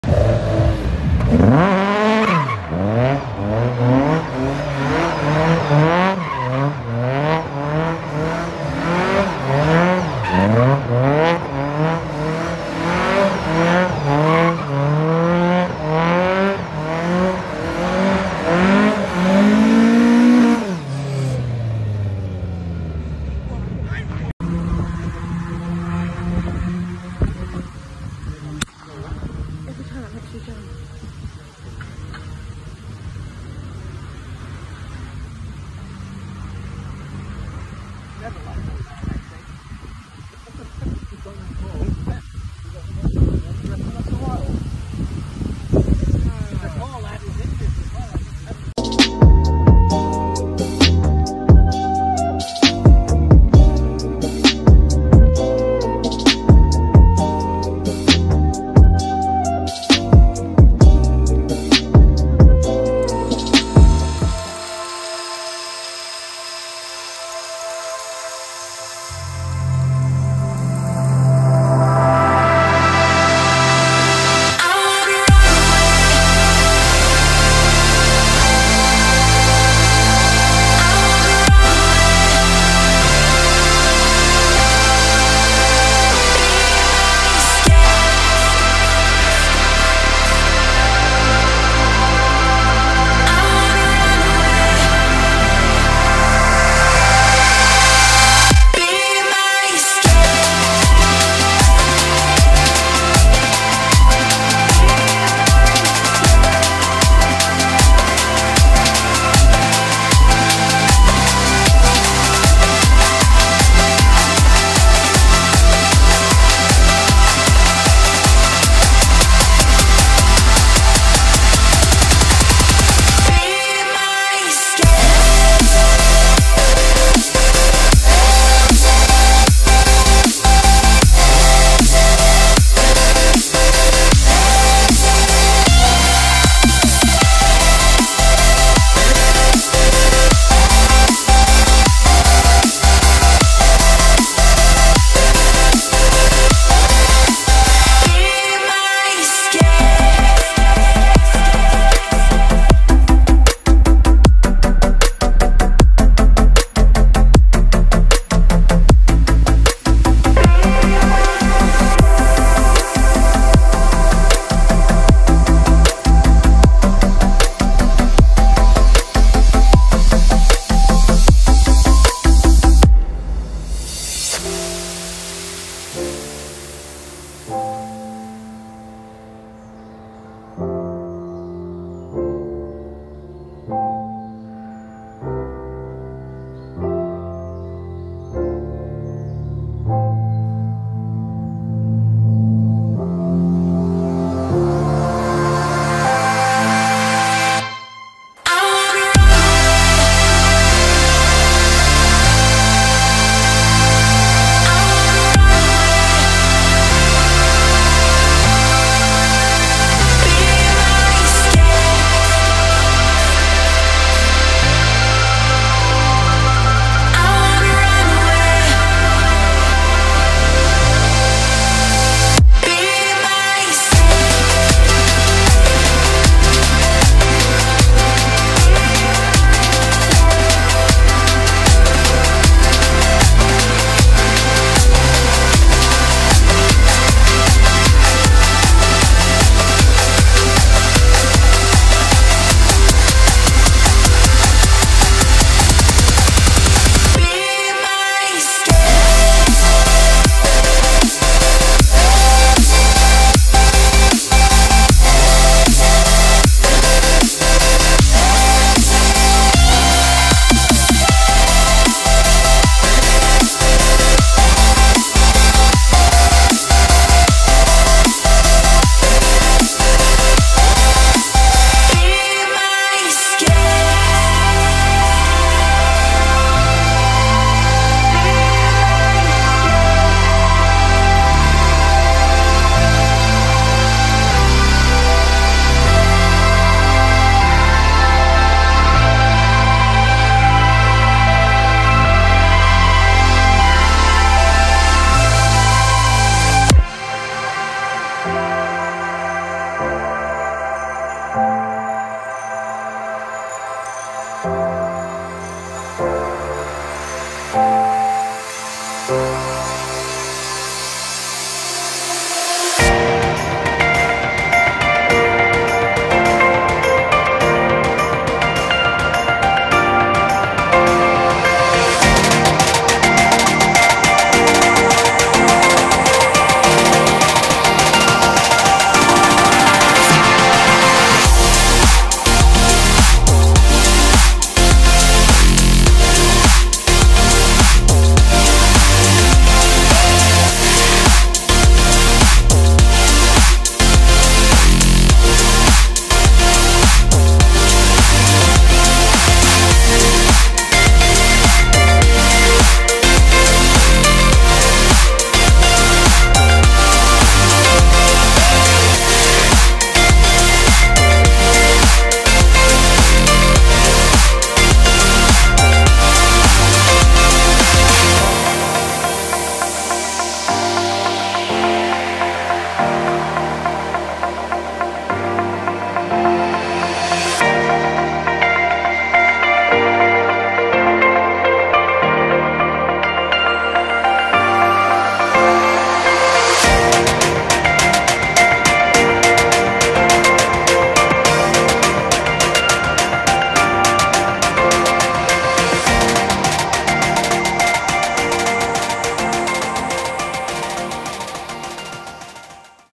Car Catches FIRE & DRIFTING At Summer Wednesdays Car Show May 22
This is mainly a clip dump of some of the cars I found cool & interesting at the first summer wednesdays meet of the year, after it being cancelled a few years ago!